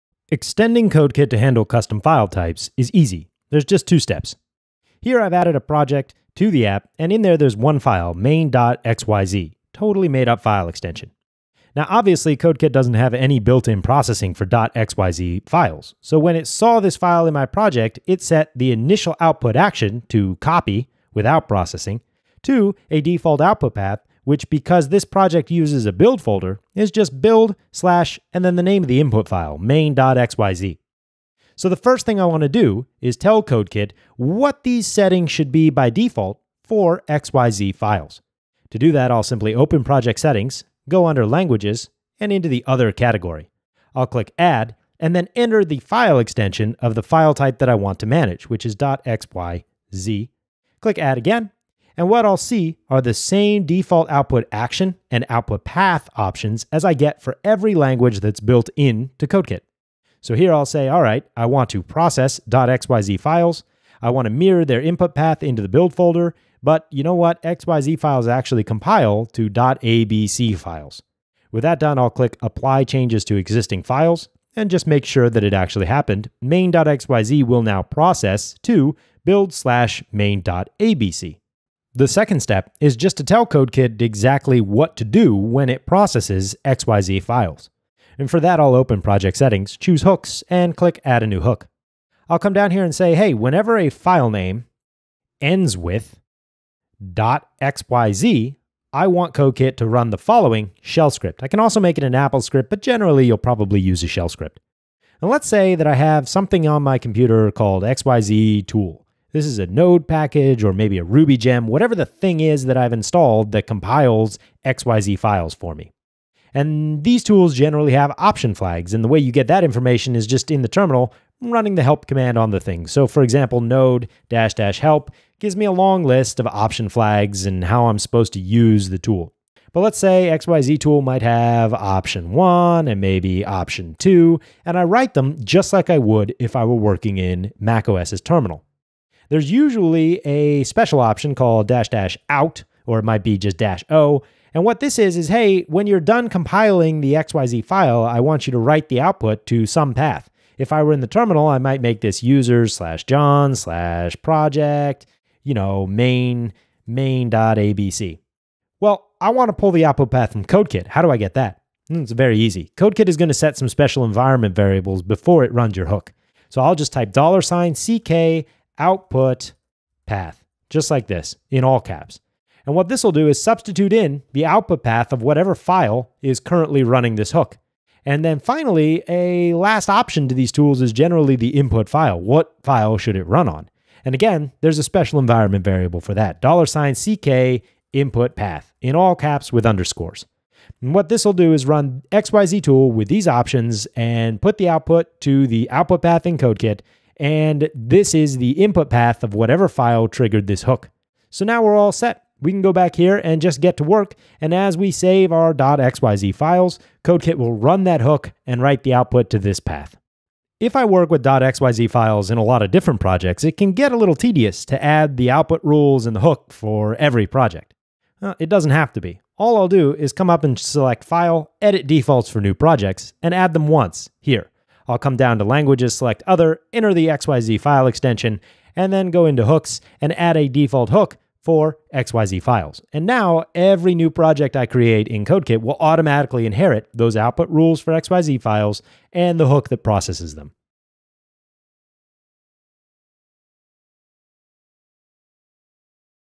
CodeKit 3: Adding Custom Languages
How to add support for any language to CodeKit 3.